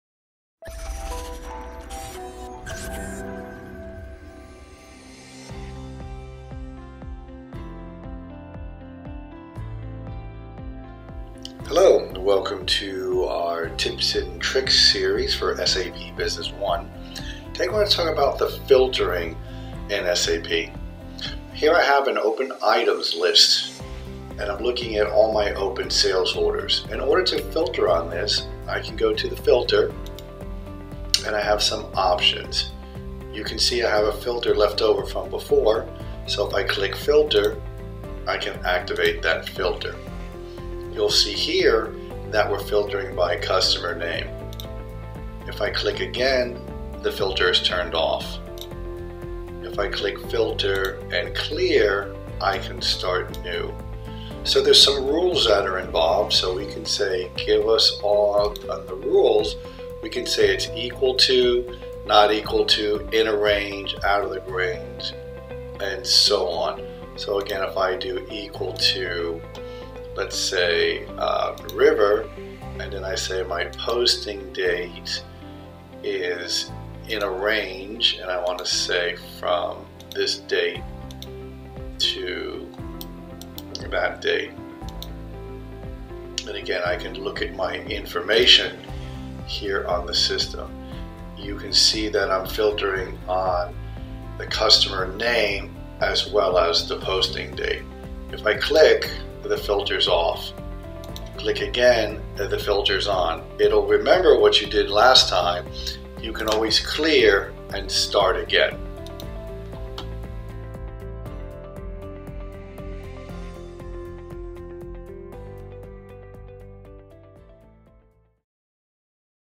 SAP Business One Consulting | Filtering Information in SAP Business One
SAP Business One Consulting for Filtering Information.
This Video Shows How to use SAP Business One to filter the information in multiple ways.
Services we provide:
SAP Business One Consulting
Process Manufacturing Software
Inventory Management Software
Warehouse Management Solutions
ERP Implementations